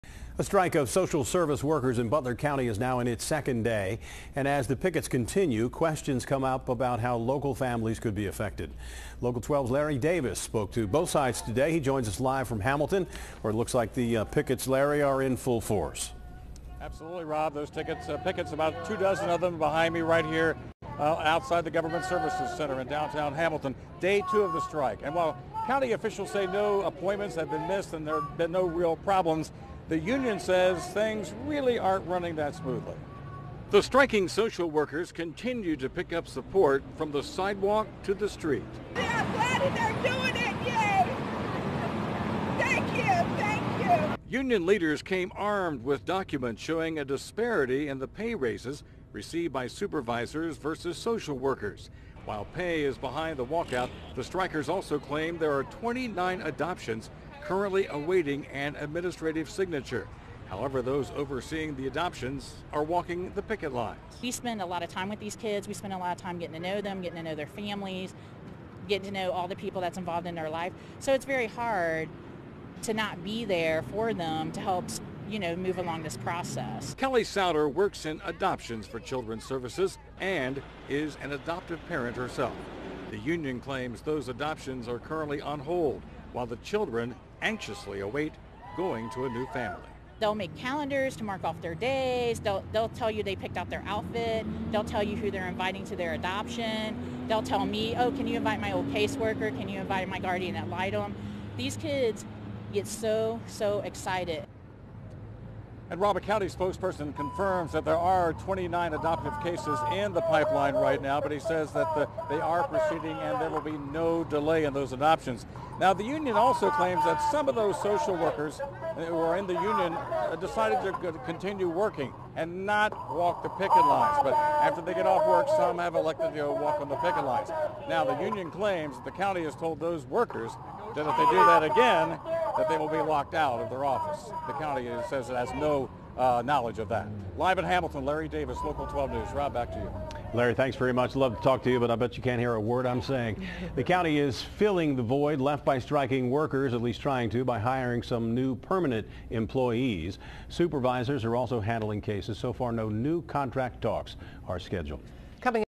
Day 2 of strike, union says dispute affectin
BUTLER COUNTY, Ohio (Larry Davis) -- A strike of social service workers in Butler County went into its second day.

As the pickets continue, questions come up about how local families could be affected.  While county officials said no appointments have been missed, the union said things weren't running that smoothly.

The striking social workers continued to pick up support from the sidewalk to the street.  Union leaders came armed with documents showing a disparity in the pay raises received by supervisors versus social workers.  While pay was behind the walkout the strikers also claimed there were 29 adoptions currently awaiting an administrative signature.  However, those overseeing the adoptions were walking the picket lines.

Kelly Sowder works in adoptions for Children Services and is an adoptive parent herself.  The union claimed those adoptions were on hold-while the children anxiously await going to a new family.

A county spokesperson confirmed there were 29 cases in the adoption pipeline but said they were proceeding and there would be no delays.

The union claimed those social workers who decided to continue working have been told not to go near the picket line or they will be locked out.  The county said it has no knowledge of that.

The county is filling the void left by striking workers by hiring some new permanent employees.  Supervisors are also handling cases.

So far no new contract talks are scheduled.

Follow Larry Davis on Twitter
@larrydaviswkrc  and LIKE him on Facebook